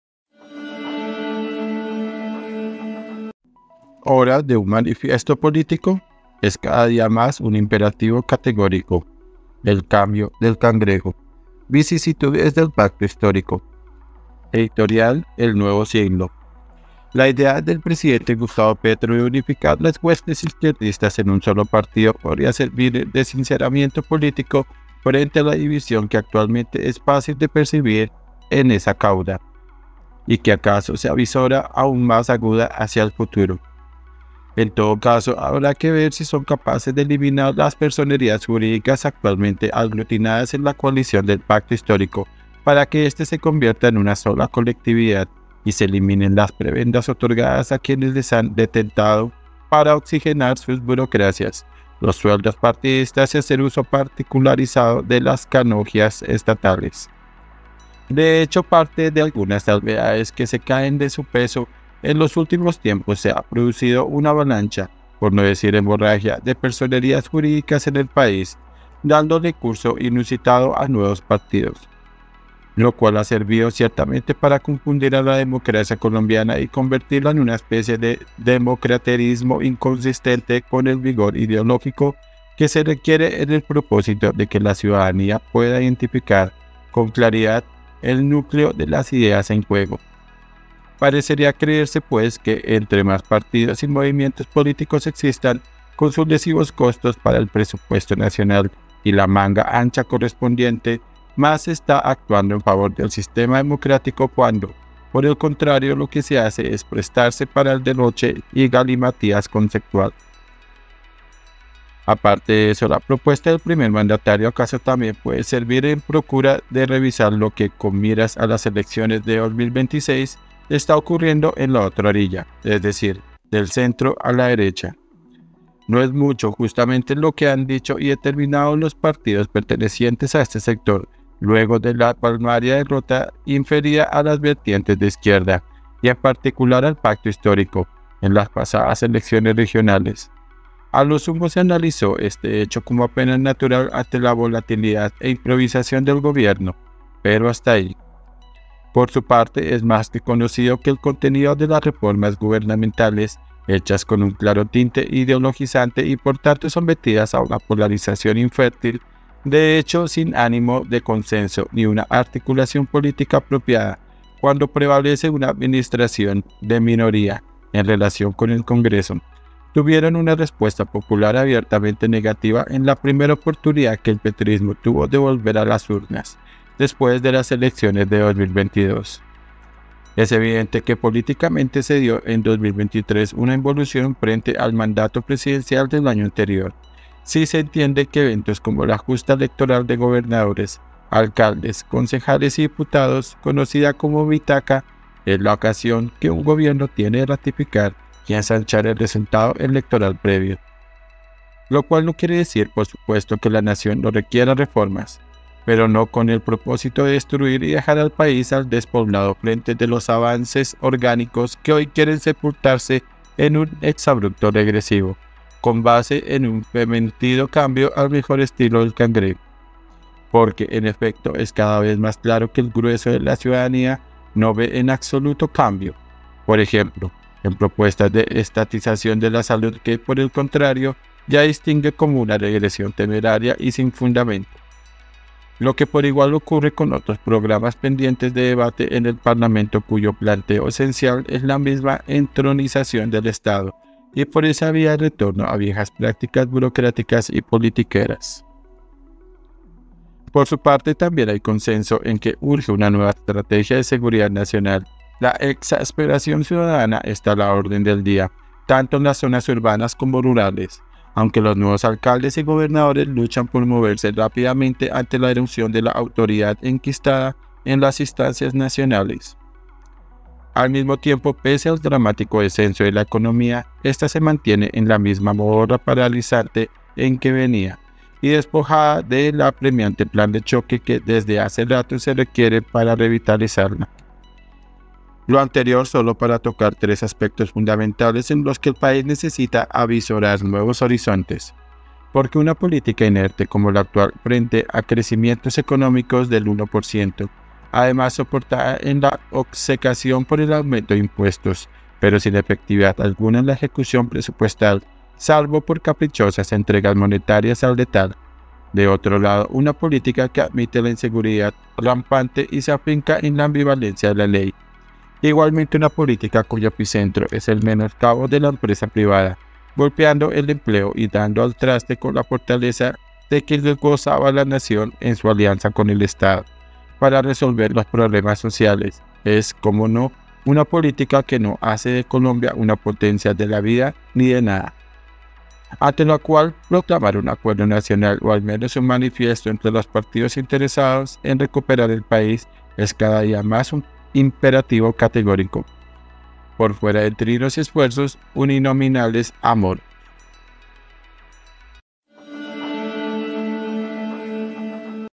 VICISITUDES DEL PACTO HISTÓRICO ¿Hora de un Manifiesto político? 🔴 Editorial El Nuevo Siglo
La idea del presidente Gustavo Petro de unificar las huestes izquierdistas en un solo partido podría servirle de sinceramiento político frente a la división que actualmente es fácil de percibir en esa cauda. Y que acaso se avizora aún más aguda hacia el futuro.